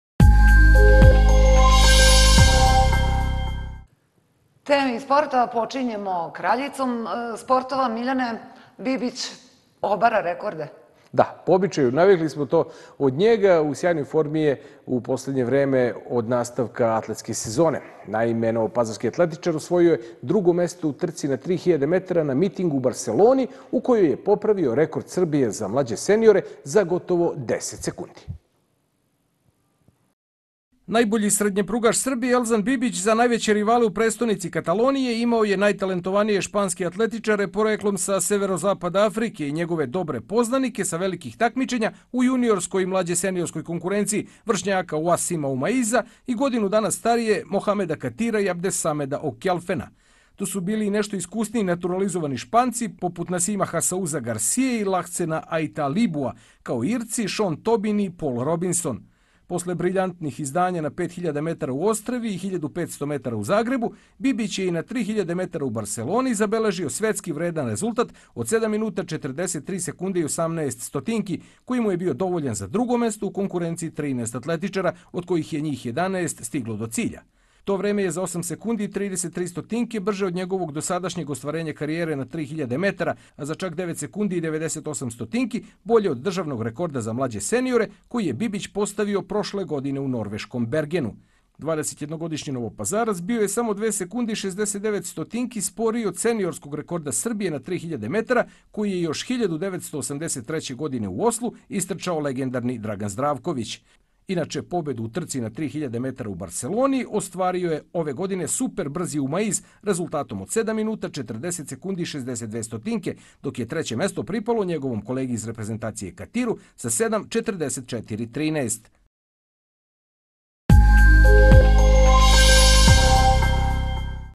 Bibić drugi u Barseloni rekordom Srbije za mlađe seniore na 3 000 metara
Novopazarski atletičar Elzan Bibić osvojio je drugo mesto u trci na 3.000 metara na mitingu u Barseloni, u kojoj je popravio rekord Srbije za mlađe seniore za gotovo 10 sekundi.